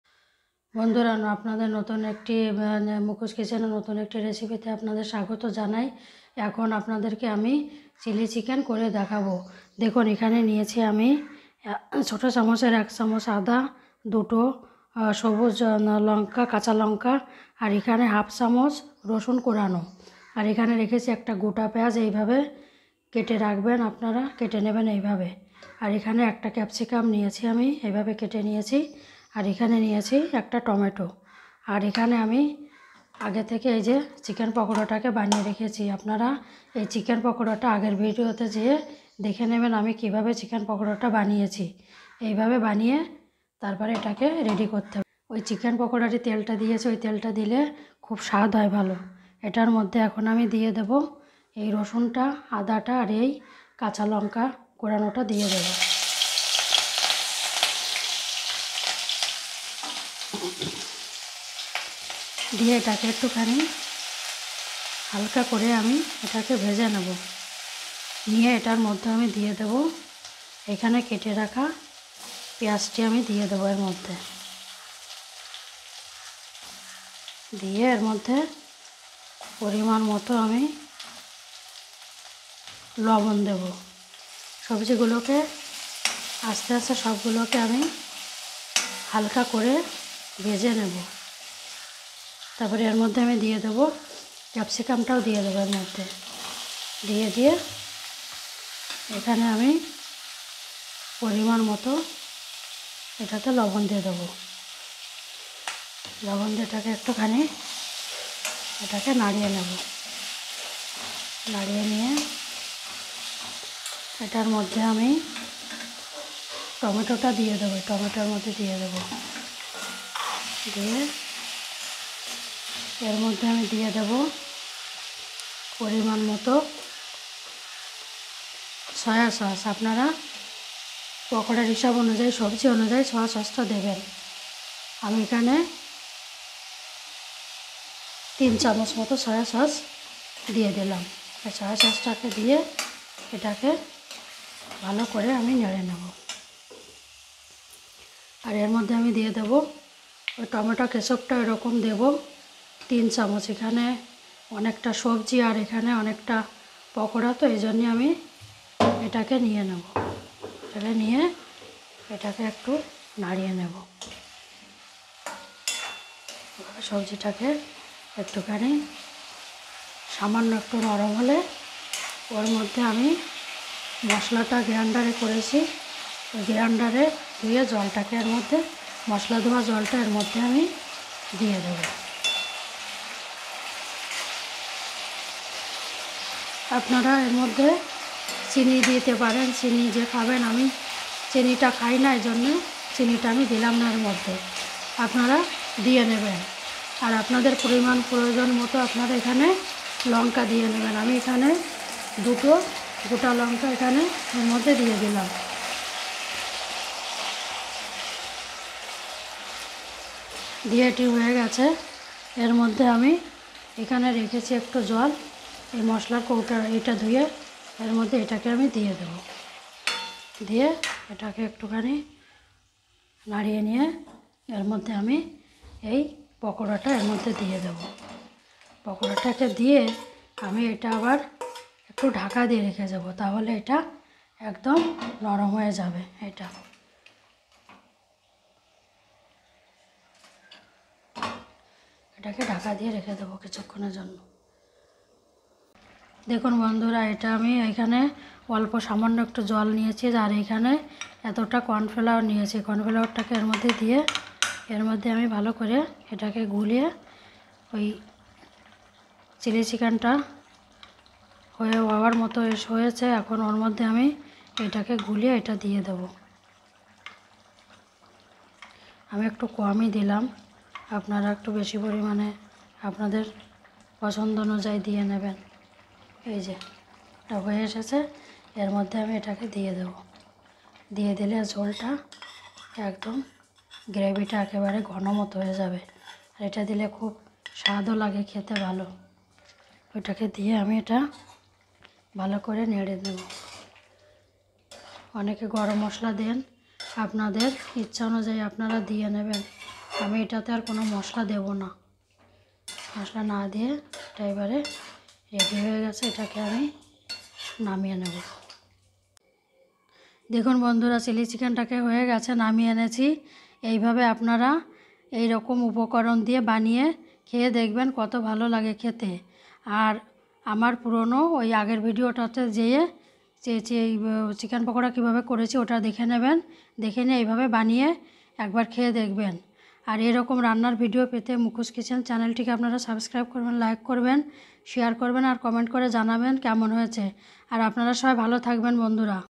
সবচেয়ে সহজ পদ্ধতিতে অনবদ্য স্বাদের চিলি চিকেন/ Mouth watering famous delicacy Chili chicken recipe
Famous dish chili chicken recipe in the most simplest way give it a try. 😊 delicious recipe bengali recipe